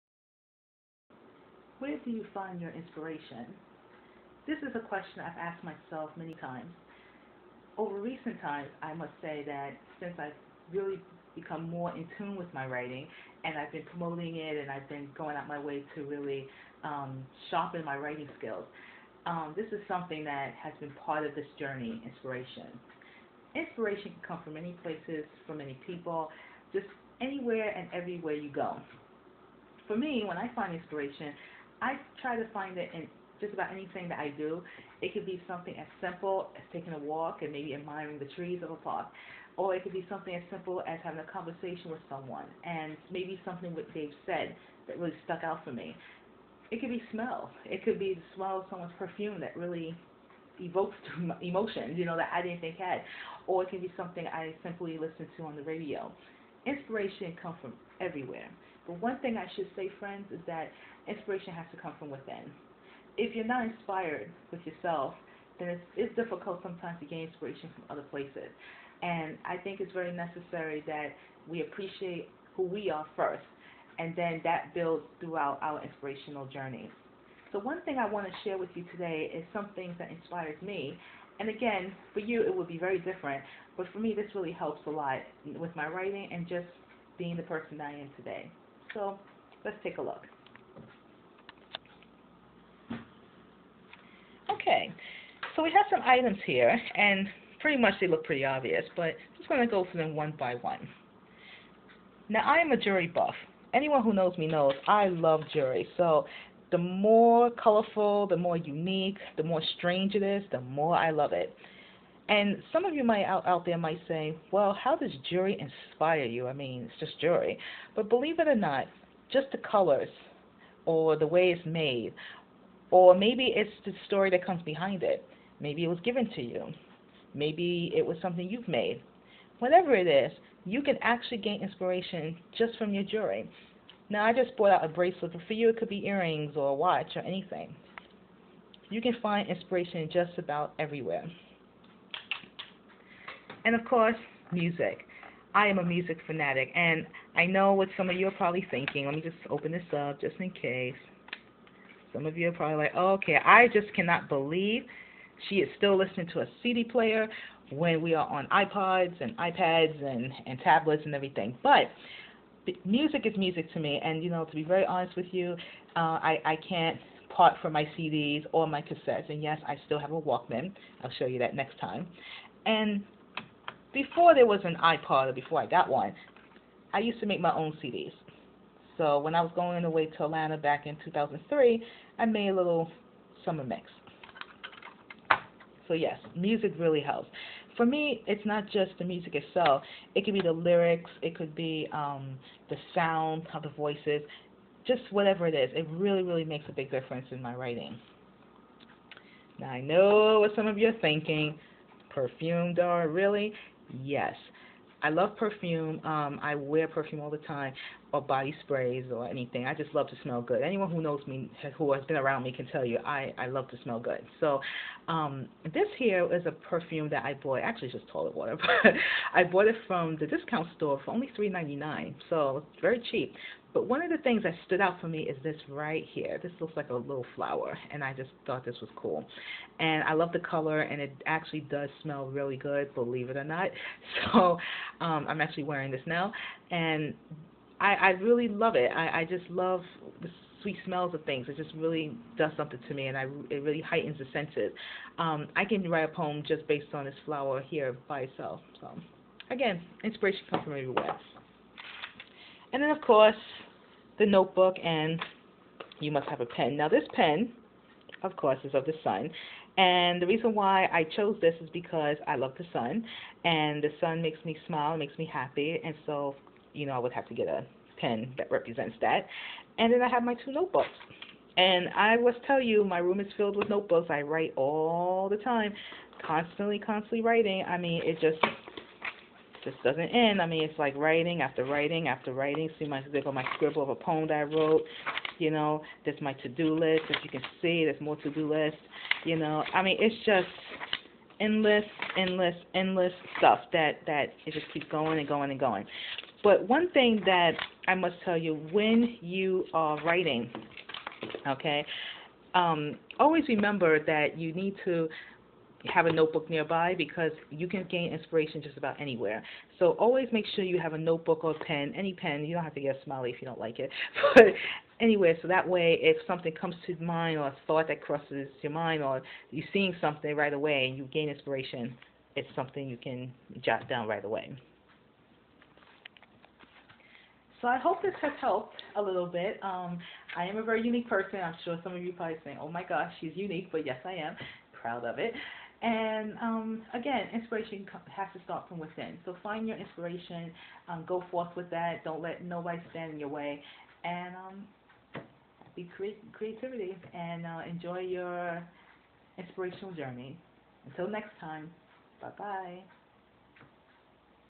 11-7-11 Finding Inspiration
Where do you find your inspiration? There are many things and elements in our world that can inspire us to feel a certain way or to do something we haven't thought of. I show you some of the things that inspires me.